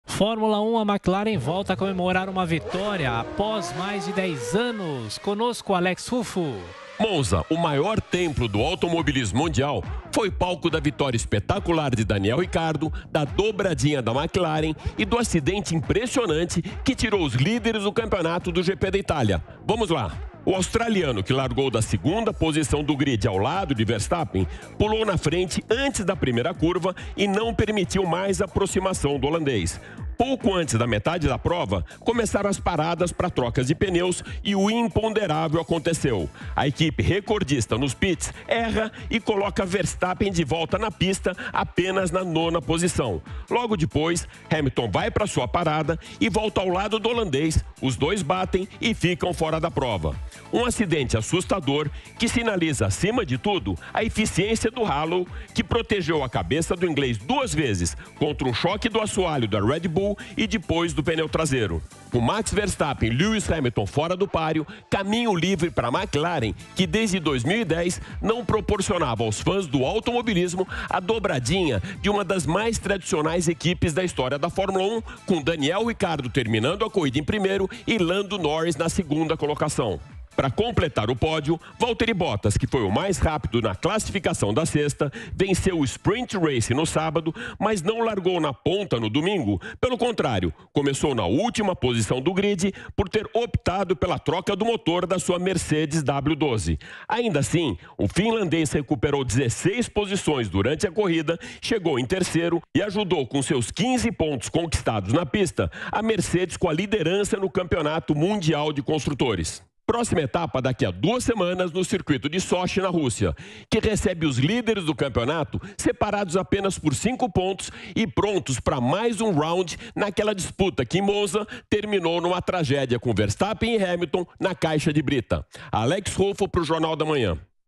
Mclaren vence em prova marcada por batida entre Verstappen e Hamilton
Monza, o maior templo do automobilismo mundial, foi palco da vitória espetacular de Daniel Ricciardo, da dobradinha da Mclaren e do acidente impressionante que tirou os líderes do campeonato do GP da Itália, Hamilton e Verstappen. Alex Ruffo traz os detalhes.